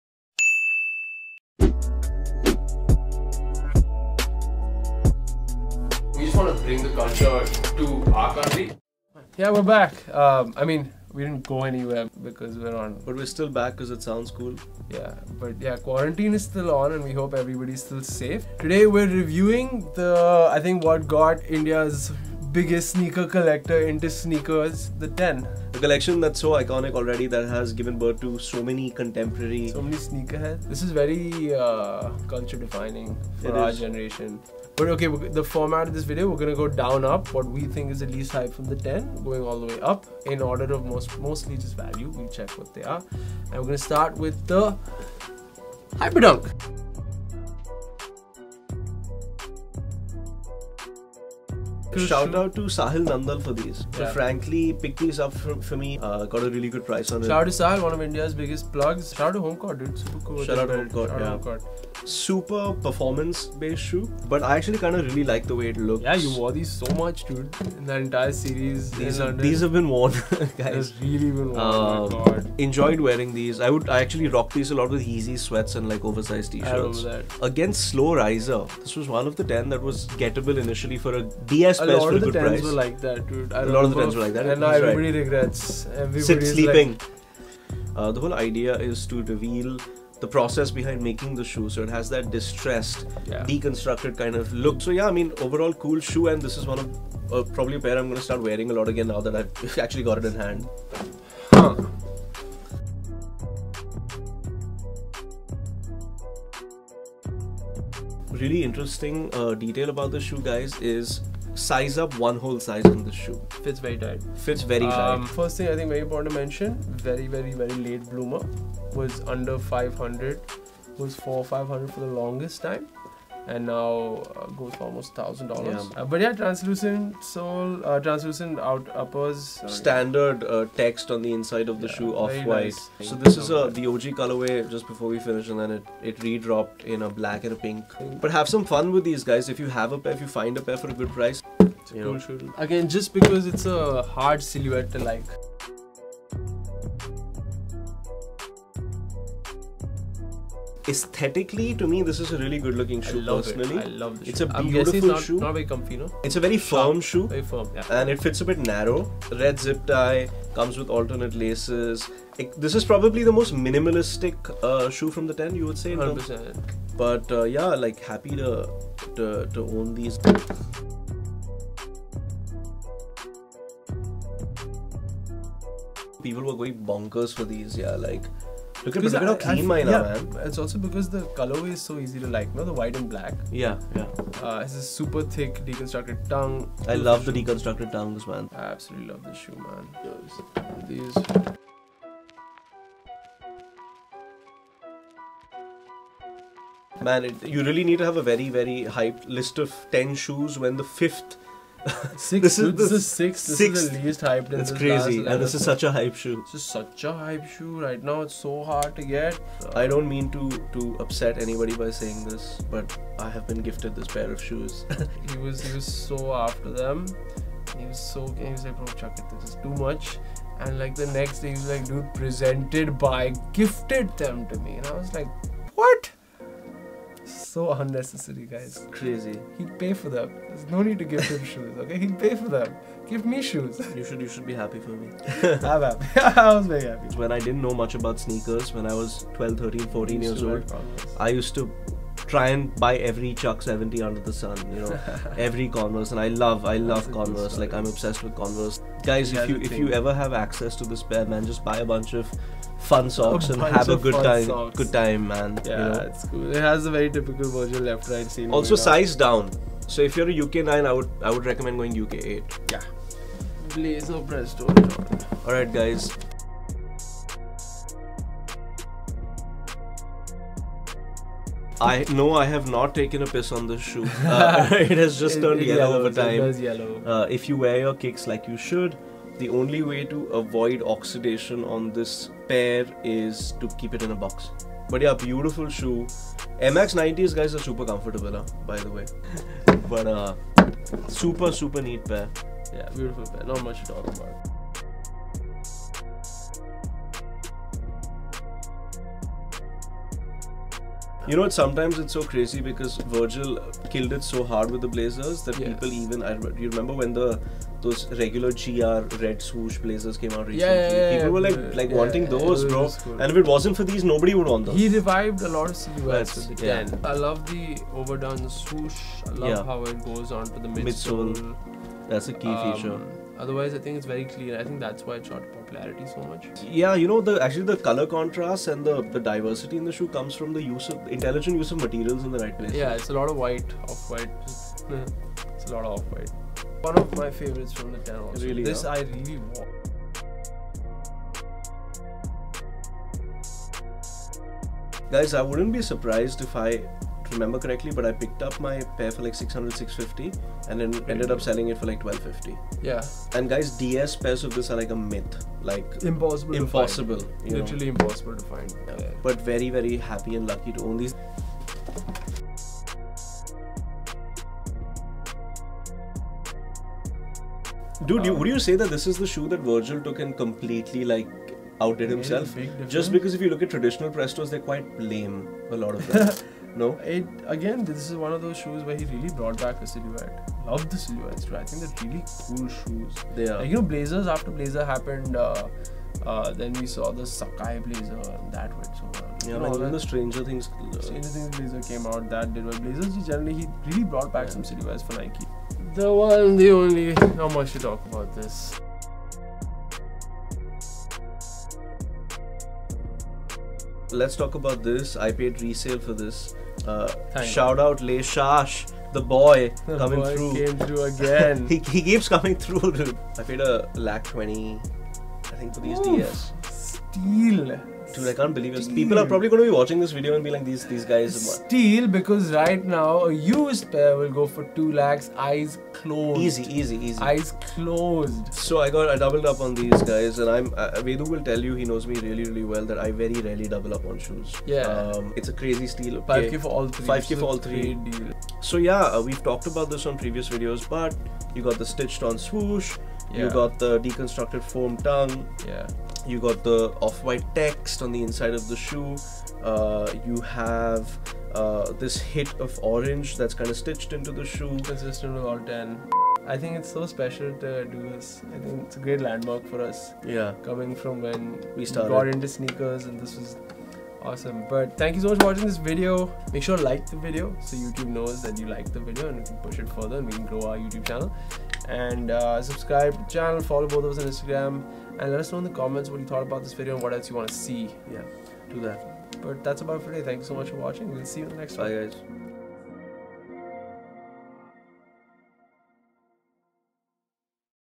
Harshvardhan Kapoor’s Off-White x Nike “The Ten” Collection in Review | MainstreetTv
Arguably one of the most iconic sneaker collaborations of modern times, Off White x Nike’s “The Ten” collection brought so many people into the game. Also being the segue to Harshvardhan’s love for collecting sneakers, we sat with him to review “The Ten” from his closet and talk about the stories behind some of them.

Follow Mainstreet : Instagram.com/mainstreettv
Follow Marketplace : Instagram.com/marketplacebymainstreet
Shop online : mainstreet.co.in